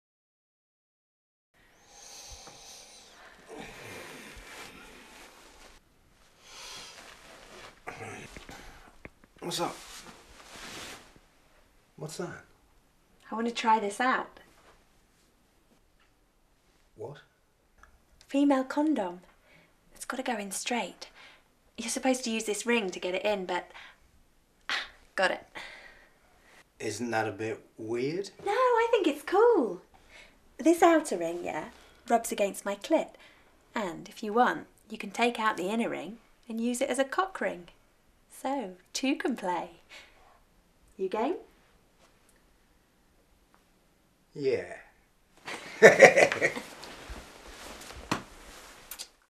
Female Condom
Information/awareness video commissioned by the International Planned Parenthood Foundation.  With British Sign Language inset.  Also subtitled (elsewhere) for Arab, Chinese, Spanish, and French viewers - we found some pretty cool translators in the process, as you can imagine!